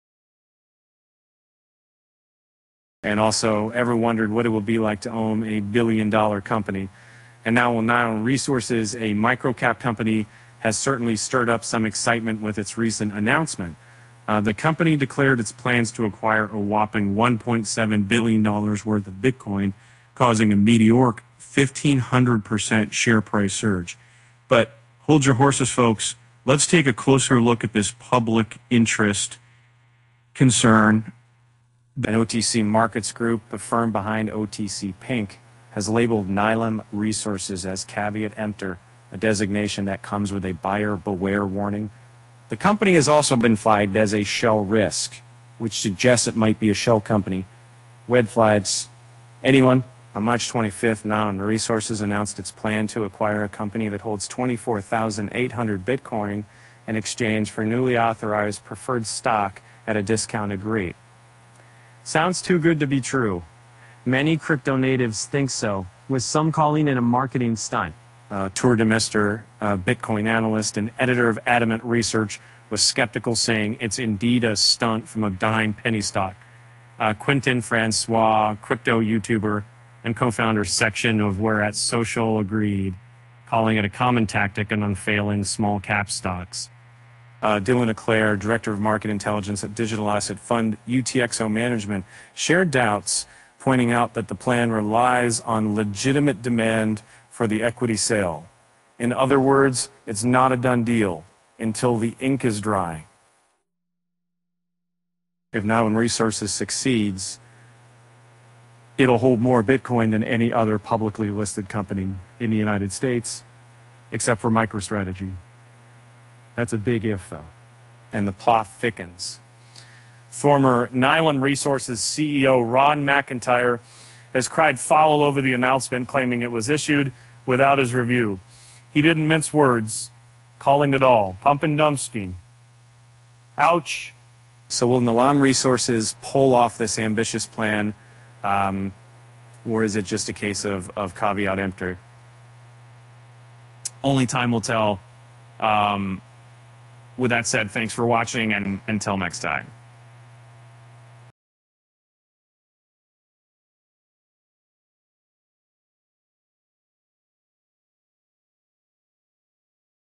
Nilam Resources flagged ‘buyer beware’ as shares pump over Bitcoin plans
Ever wondered what it would be like to own a billion-dollar company? Well, Nilam Resources, a micro-cap company, has certainly stirred up some excitement with its recent announcement. The company declared its plans to acquire a whopping $1.7 billion worth of Bitcoin, causing a meteoric 1,500% share price surge! But, hold your horses, folks. Let's take a closer look at this "public interest concern."